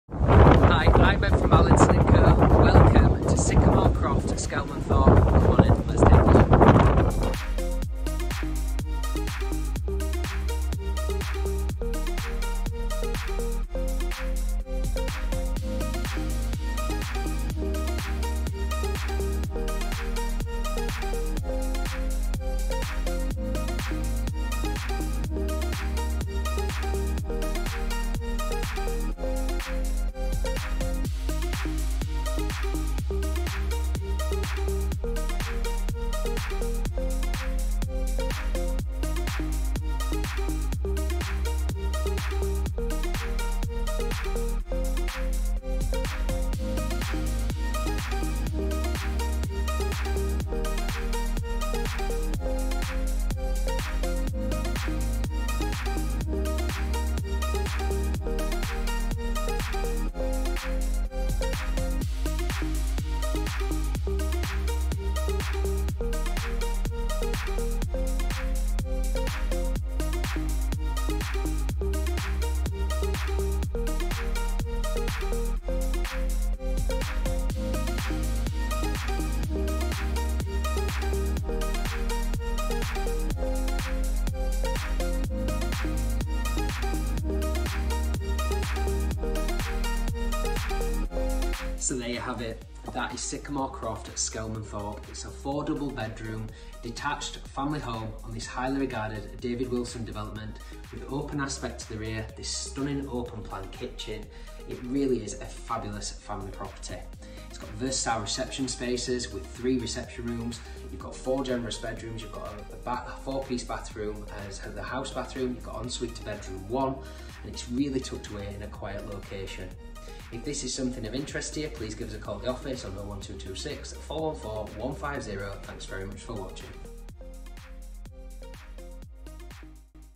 Sycamore Croft, Skelmanthorpe, Huddersfield, HD8 9UX
WOW, WOW, WOW, … A SIMPLY STUNNING, FOUR DOUBLE BEDROOM FAMILY HOME, SET UPON THIS HIGHLY REGARDED DAVID WILSON DEVELOPMENT, FEATURING THREE RECEPTION ROOMS, OPEN PLAN KITCHEN, GARAGE, OFF STREET PARKING AND A PRIVATELY ENCLOSED REAR GARDEN WITH A PLEASANT ASPECT.